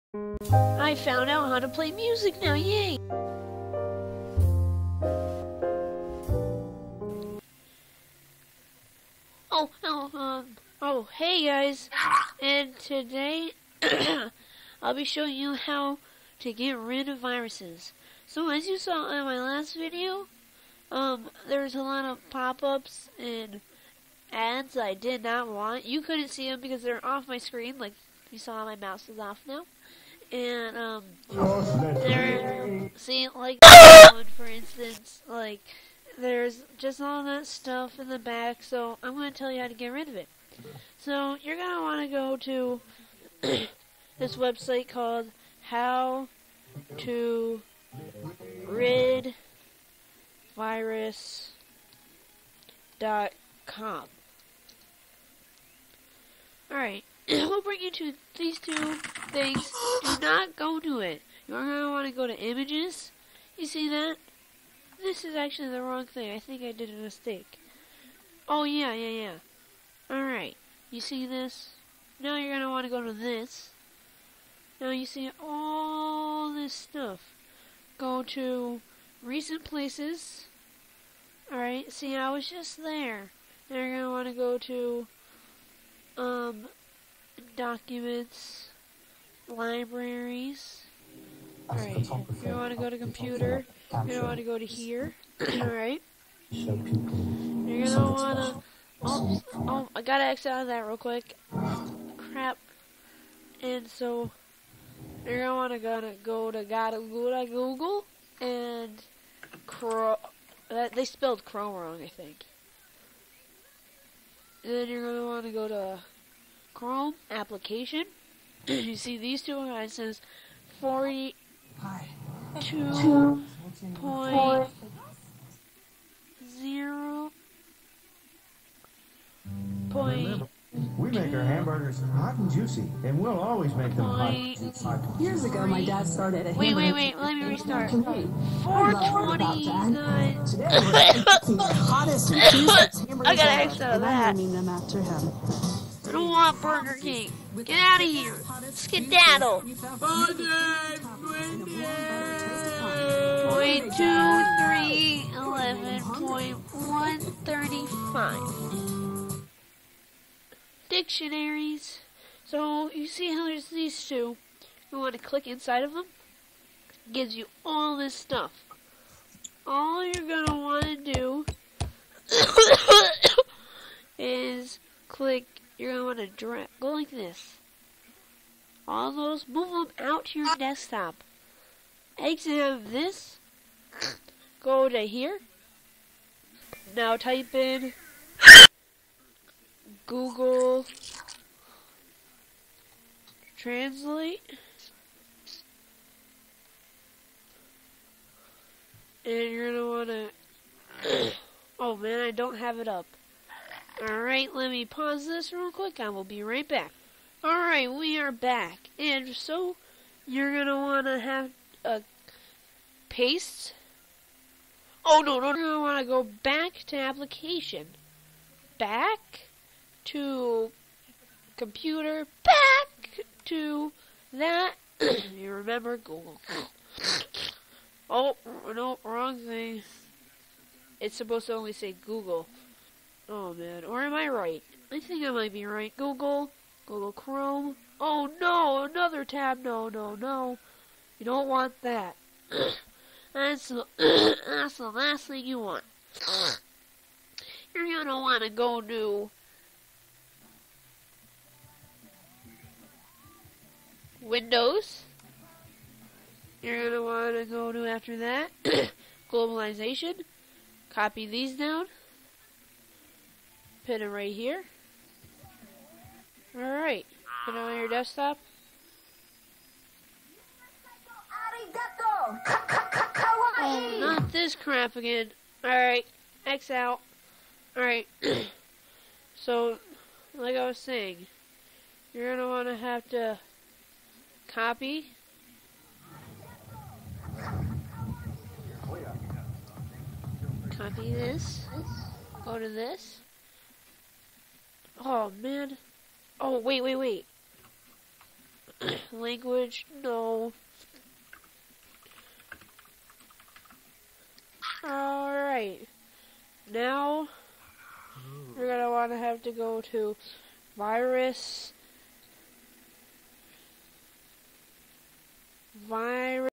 Tutorial | How to get rid of viruses on your computer for free!
In this tutorial I will show you how to get rid of all of your Trojans, Male-ware, spy-ware viruses and other stuff like that with only the click of a button. This will be a little complicated at first but once you get into it.. it won't be that hard. And you'll also have a faster computer speed and no more pop-up ads and stuff like that.